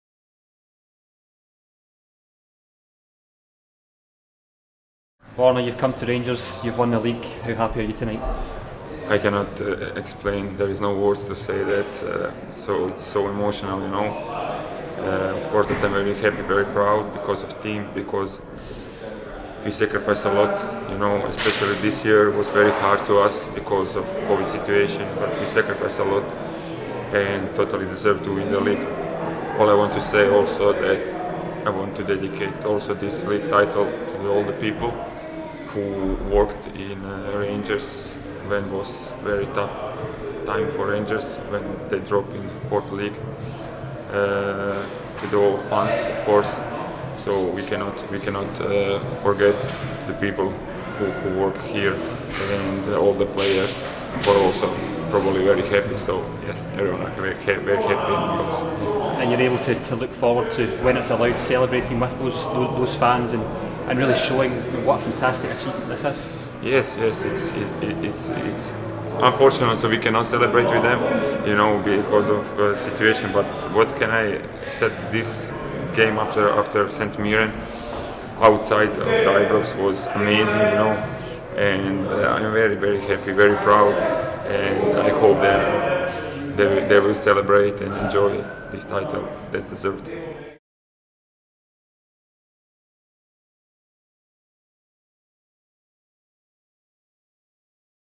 Champion55 | Borna Barisic
Borna Barisic speaks to RangersTV after clinching title number 55 for Rangers.